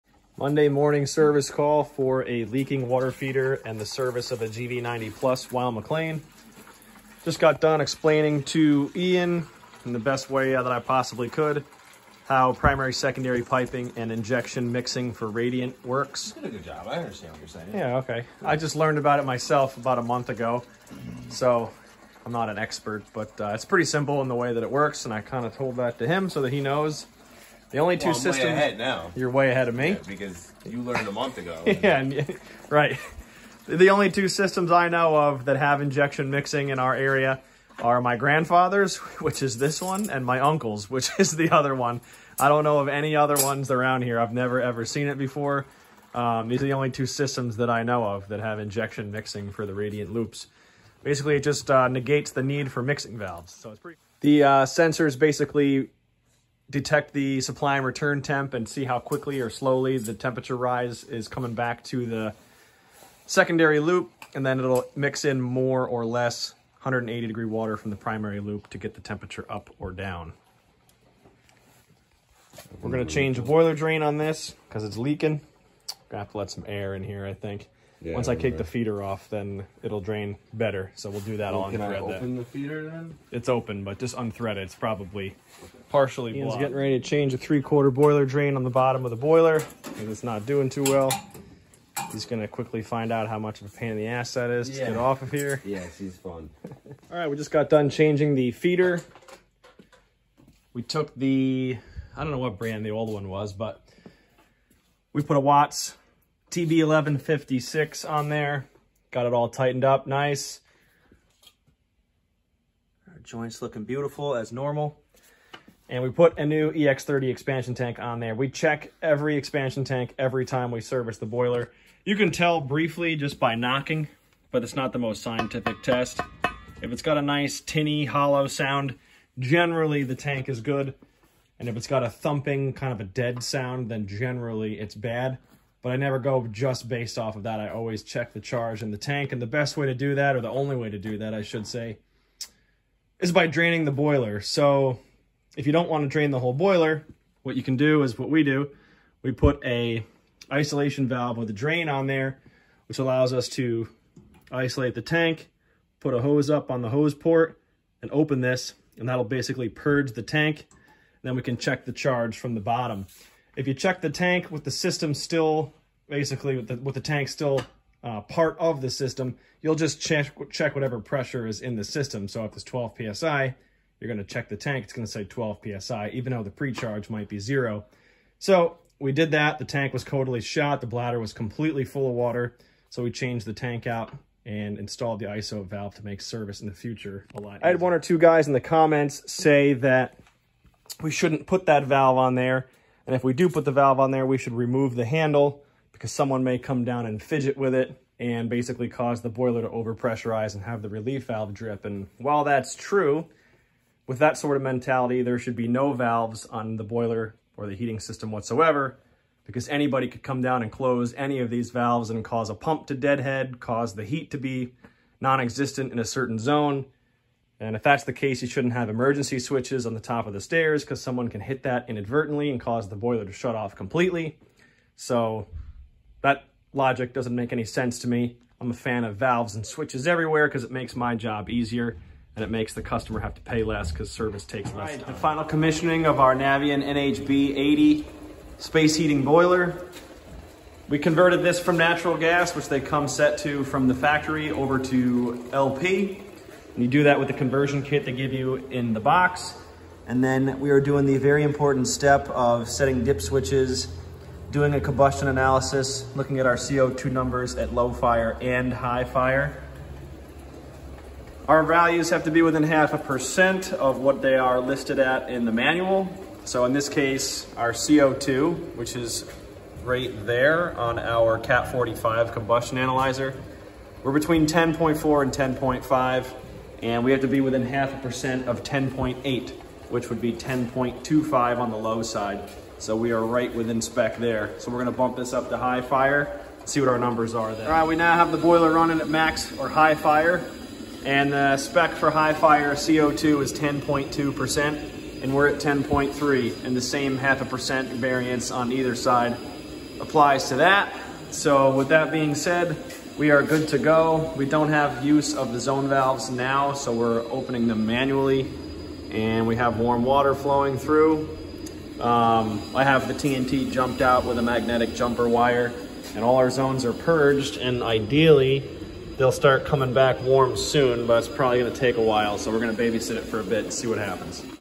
Heating Boiler Maintenance and New Navien Startup! Radiant Floor Heating!
Come song as Ian and I clean, maintain and test a 15 year old Weil McLain GV90+ heating boiler, as well as perform the initial startup on a brand new Navien NHB-80 wall hung condensing boiler!